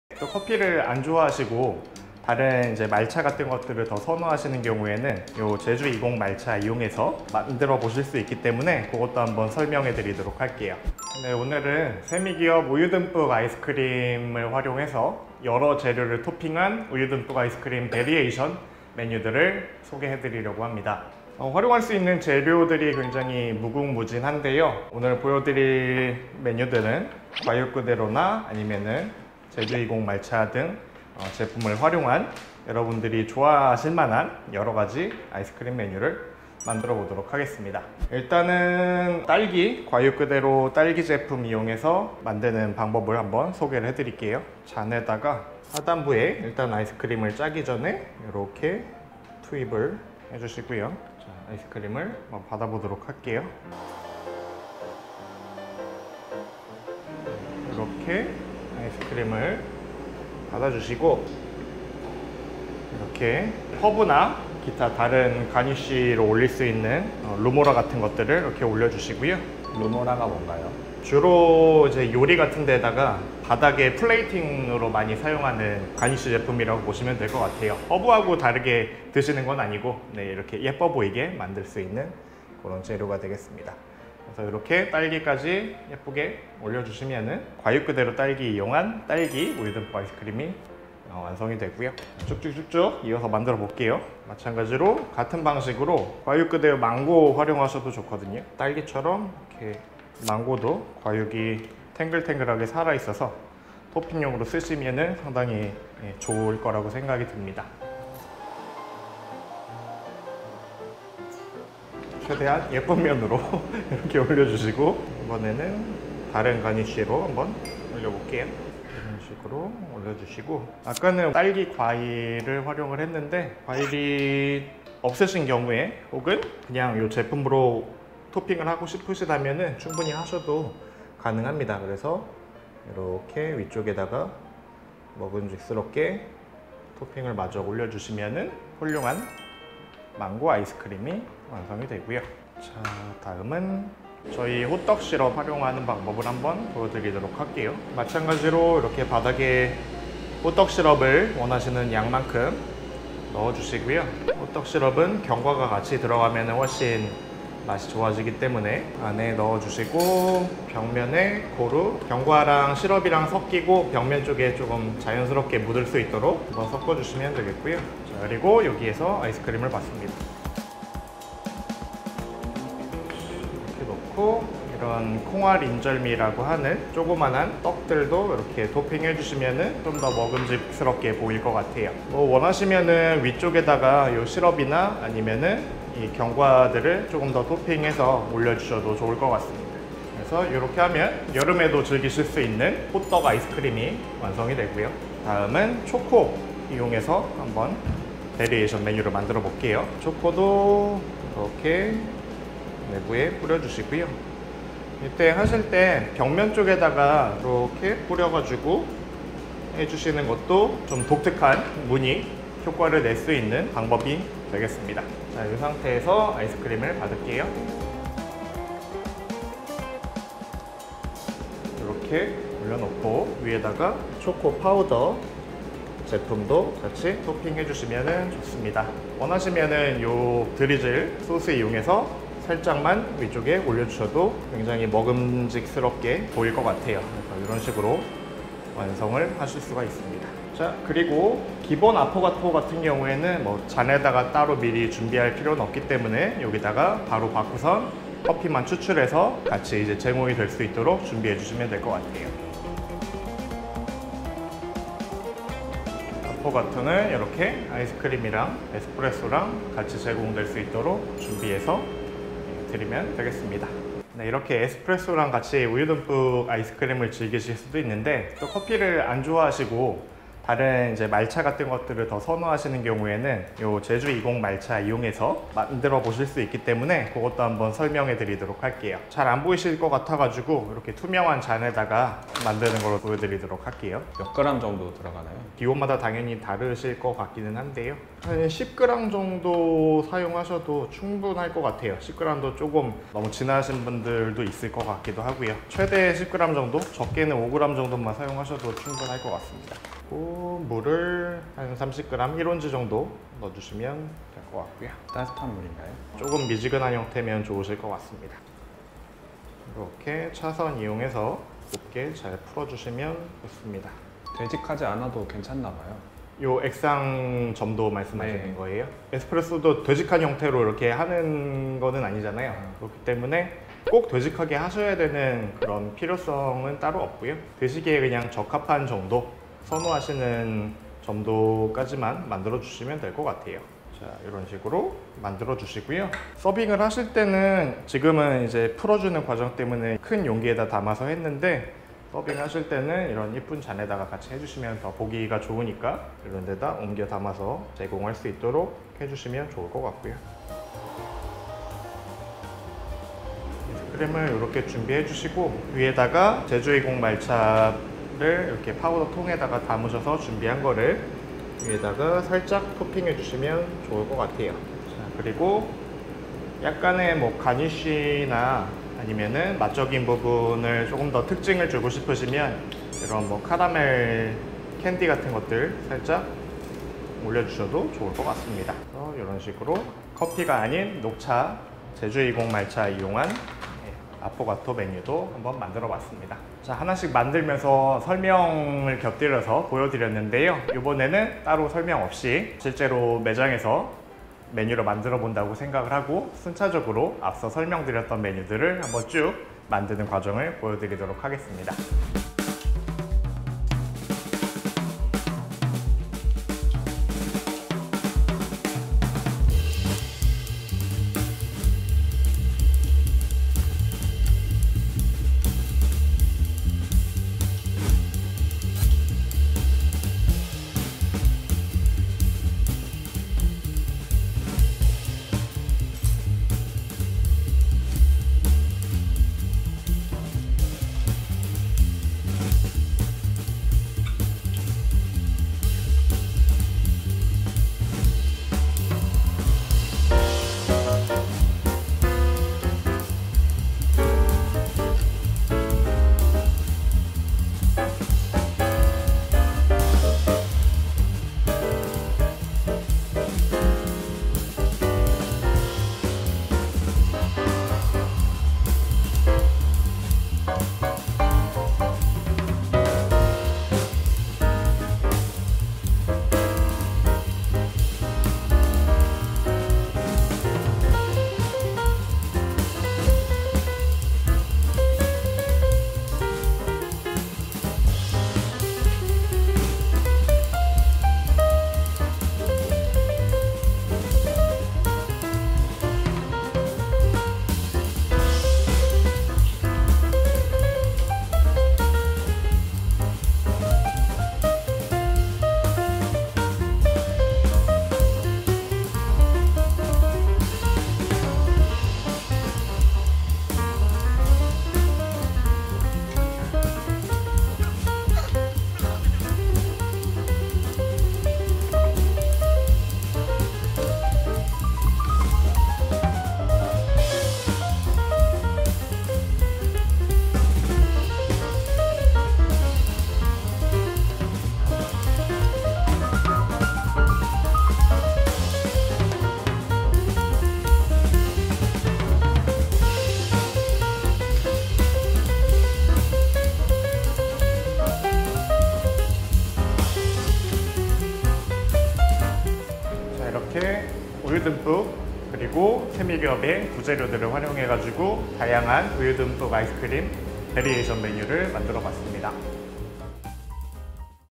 아포가토에 커피를 대신할 베이스&토핑재료 5종 추천
칼피자니 머신으로 추출한 소프트 아이스크림에 에스프레소만 부어주시면 아포가토가 되는데요!

세미기업의 베버시티 제품을 활용해 커피를 대신할 재료 5종을 소개합니다.